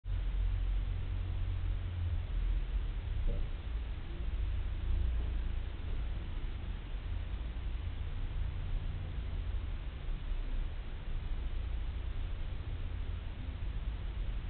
Atmung der Amsel
Es beeindruckt mich immer wieder wie deutlich man eine Amsel atmen sehen kann. Und das in einer Ruhephase. Wie heftig wird die Atmung erst sein, nach einem Flug?